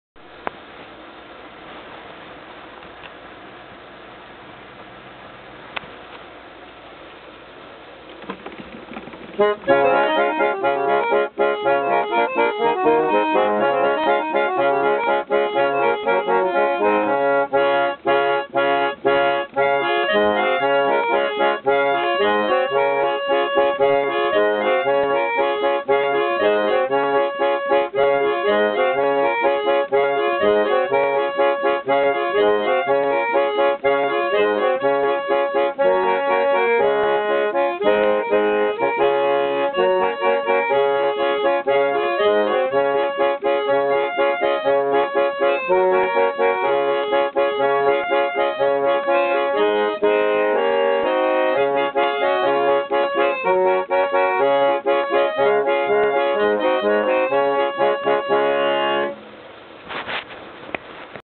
SYMPHONIA ORGANETTE
Symphonia 20 note pneumatically operated organette produced by the Wilcox & White Organ Co.